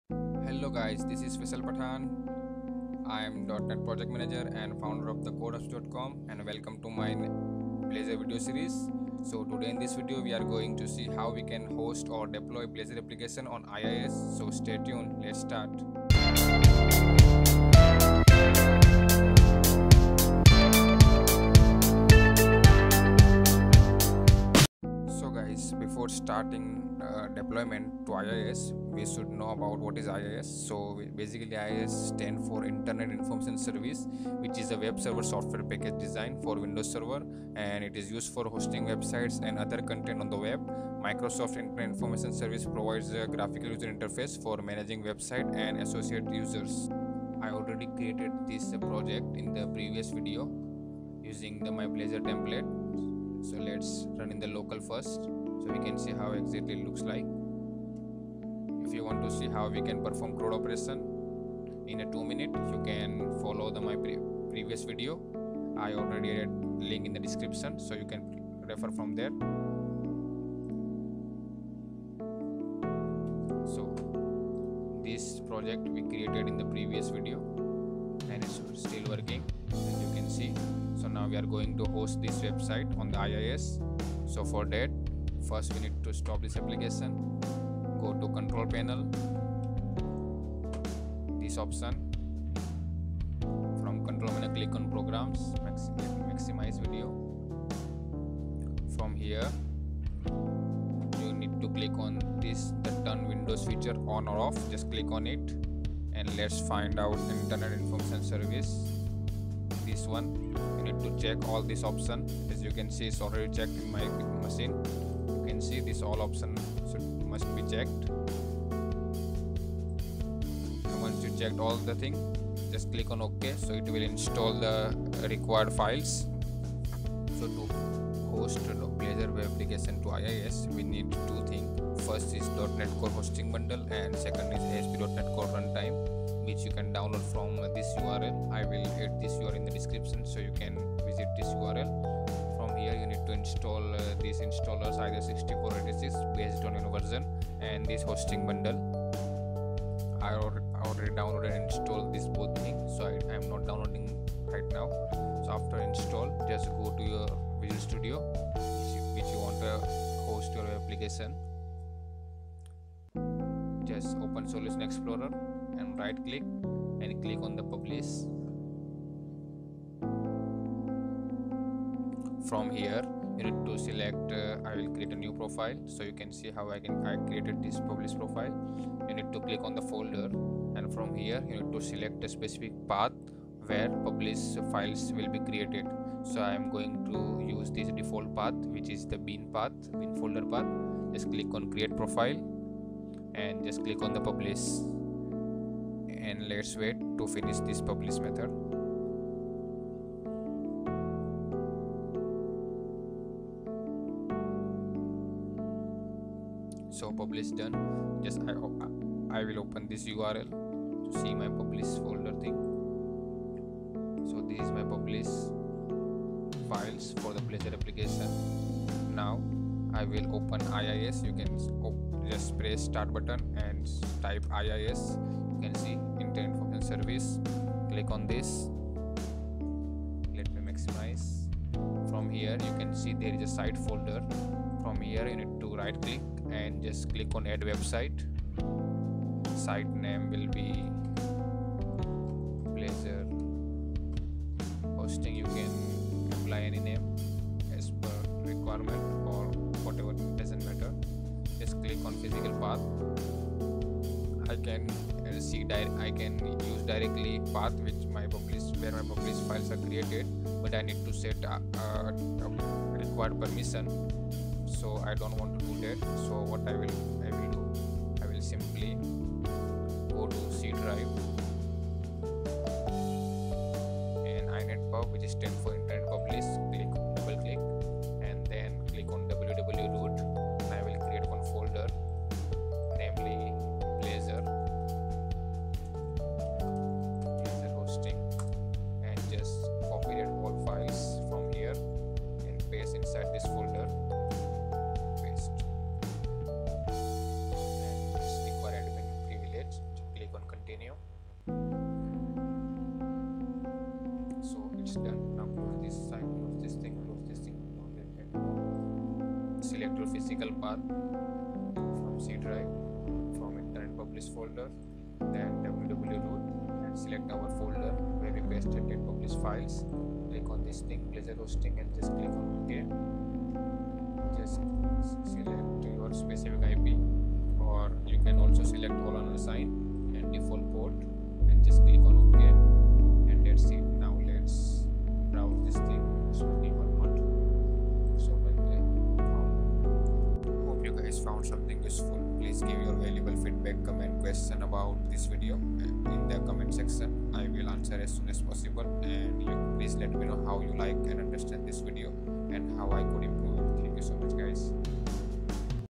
Deploy Blazor application in IIS
I will deploy a Blazor application in IIS on with Windows OS.

To download
- NET Core hosting bundle
- ASP.NET Core Runtime